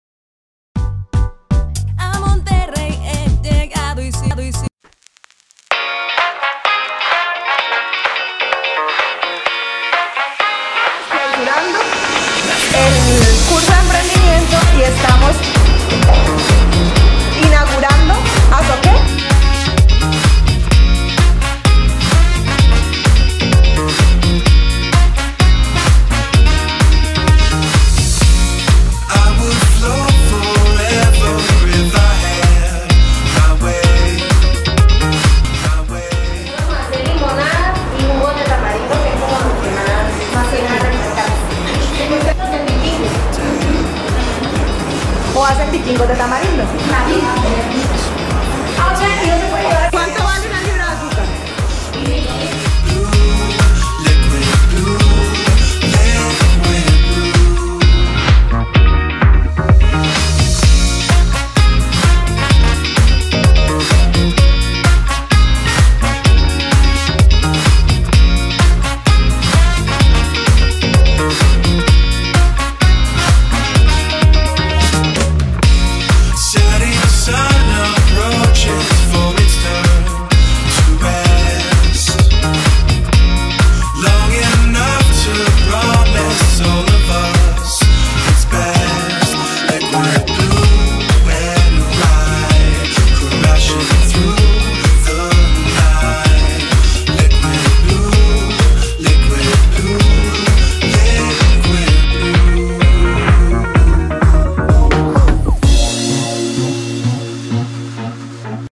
Clausura Curso de emprendimiento
¡Así terminó nuestro curso de emprendimiento con las esposas y familiares de nuestros trabajadores en Puerto Wilches!